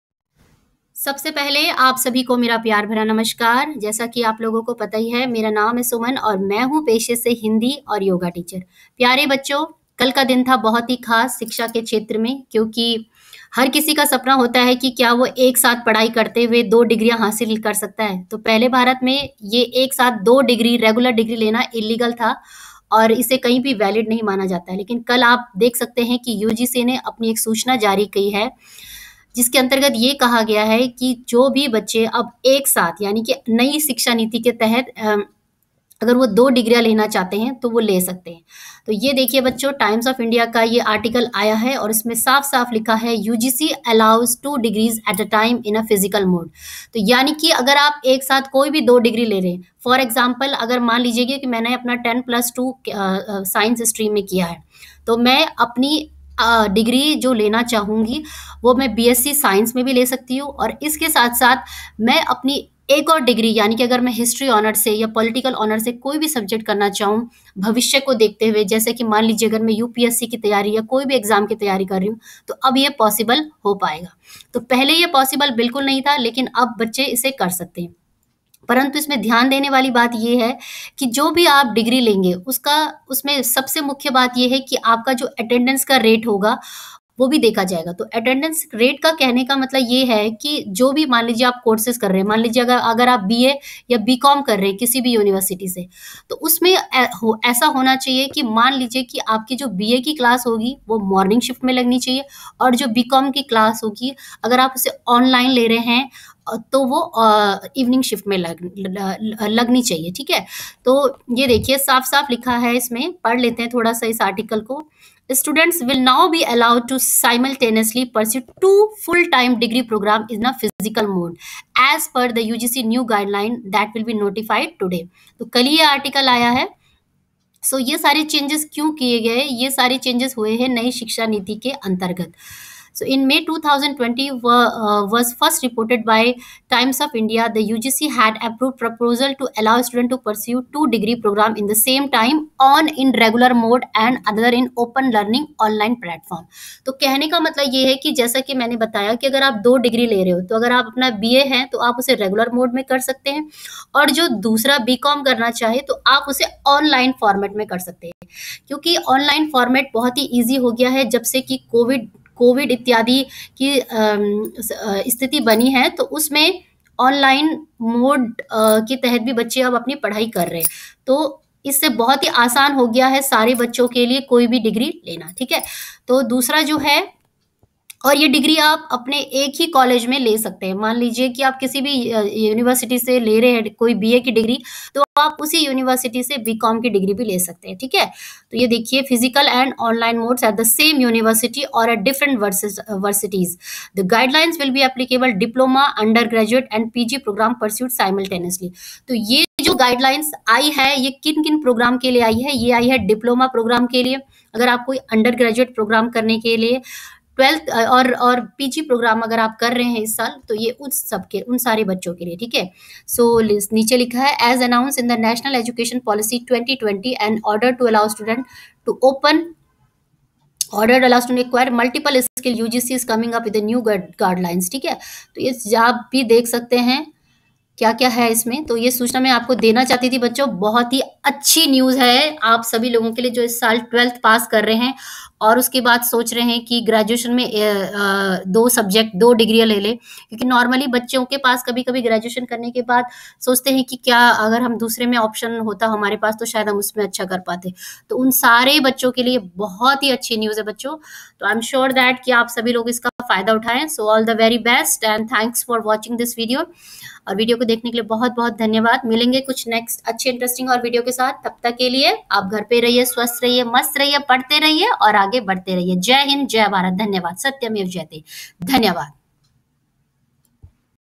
UGC Allows 2 Degree Simultaneously lअब होगा (2 - दो) Degree लेना होगा आसान एक साथ l UGC Latest News
In today's video , I will explain you UGC Latest Update.

Teaching & Learning Hub.
# Two degree simultaneously is valid now in India.
# can i pursue tow degree together